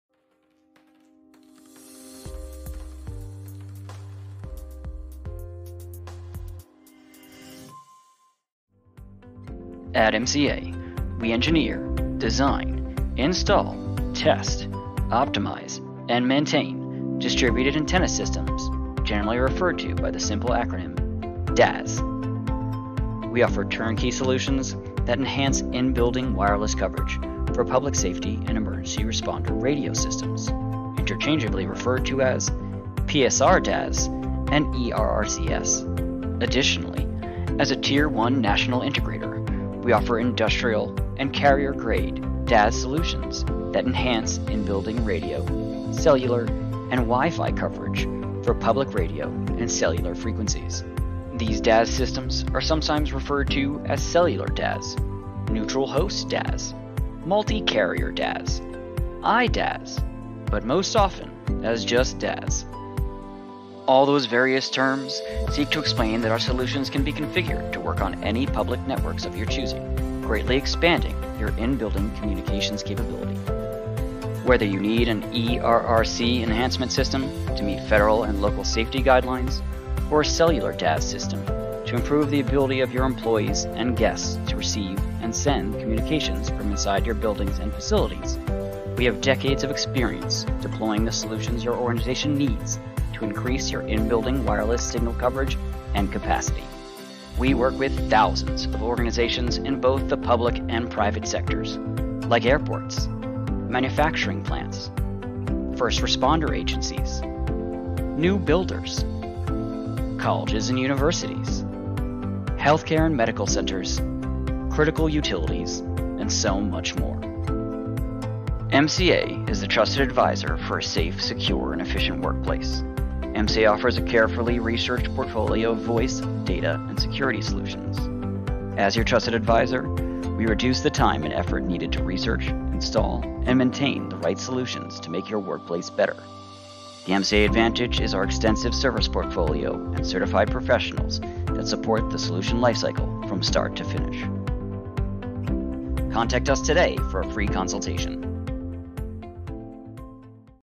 MCA DAS Solutions Overview
DAS Solutions for In-Building Wireless Coverage
As experts in the engineering, design, installation, testing, optimization, and maintenance of Distributed Antenna Systems (DAS), MCA is committed to a level of unmatched personalized service and support. We offer turnkey solutions for Emergency Responder Radio Coverage Systems (Public Safety DAS for ERRCS), Radio DAS solutions that enhance in-building UHF and VHF signal coverage, and Cellular DAS solutions to improve in-building LTE and 5G coverage.

The materials used in the construction of your facilities can block cellular and radio signals. Our solutions help ensure your buildings remain compliant with local fire codes, cellular signals never drop due to poor in-building signal coverage, and that your work teams have the radio coverage they need to effectively perform their jobs. Nearly every industry needs the in-building wireless coverage solutions MCA has to offer.